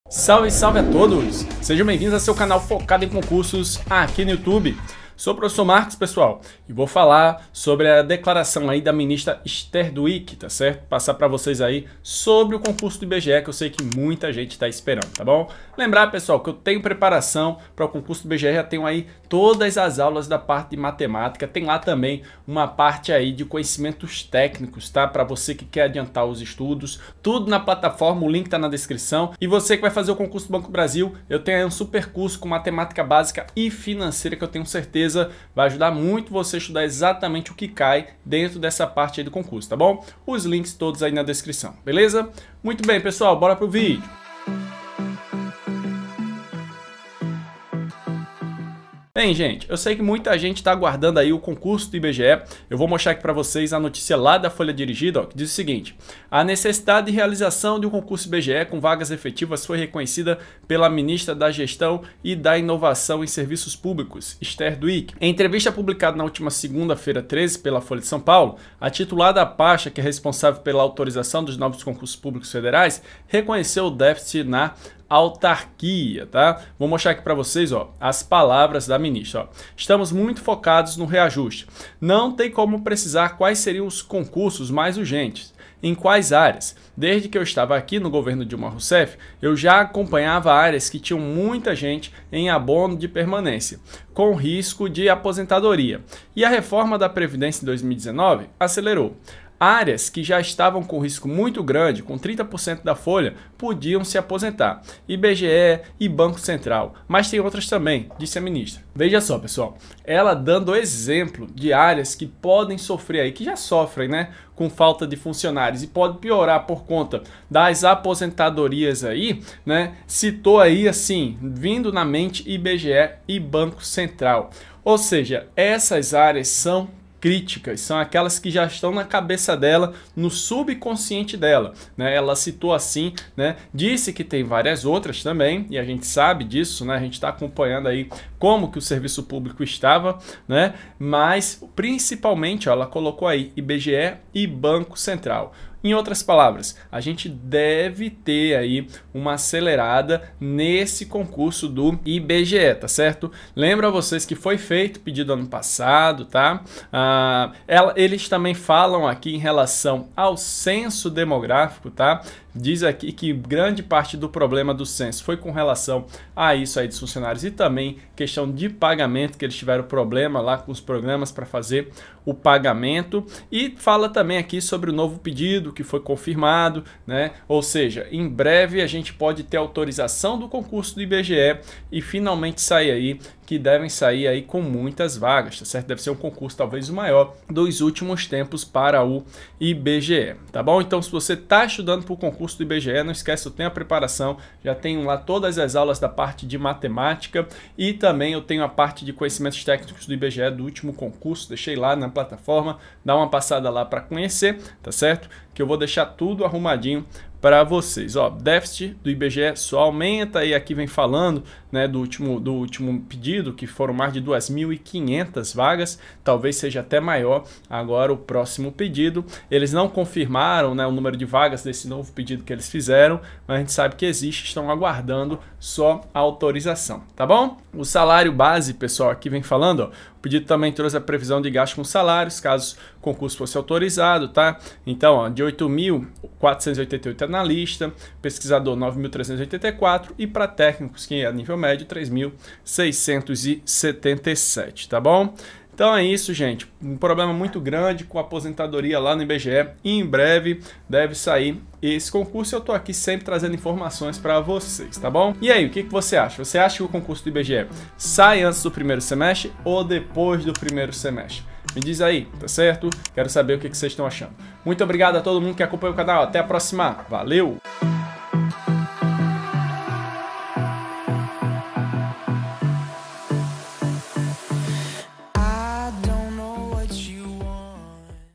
Urgente!!! Novo Concurso IBGE 2023 Efetivos !!! Ministra declarou !!! Editais 2023.
A necessidade de realização de um concurso IBGE, com vagas efetivas, foi reconhecida pela ministra da Gestão e da Inovação em Serviços Públicos, Esther Dweck.

Em entrevista publicada na última segunda-feira, 13, pela Folha de S.Paulo, a titular da pasta, que é responsável pela autorização dos novos concursos públicos federais, reconheceu o déficit na autarquia.

Segundo Esther Dweck, ainda não é possível confirmar quais órgãos receberão aval para novos concursos em 2023. No entanto, ela reconhece que, desde o governo Dilma Rousseff, há áreas com risco muito grande, tendo 30% da folha apta à aposentadoria.

"Estamos muito focados no reajuste. Não tem como precisar quais seriam os concursos (mais urgentes), em quais áreas. Desde que eu estava aqui (no governo Dilma Rousseff), eu já acompanhava áreas que tinham muita gente em abono de permanência, com risco de aposentadoria, e a reforma da Previdência em 2019 acelerou. Áreas que já estavam com risco muito grande, com 30% da folha podiam se aposentar. IBGE e Banco Central, mas tem outras também", disse a ministra.

Ao citar o Instituto Brasileiro de Geografia e Estatística, a ministra reconhece que há uma necessidade de servidores efetivos na autarquia.

Exemplo disso está no adiamento do Censo 2022. Prevista para ser finalizado no ano passado, a coleta ainda está em andamento e só deve ser concluída, segundo o IBGE, em abril deste ano.Os sucessivos atrasos ocorreram devido à falta de pessoal temporário.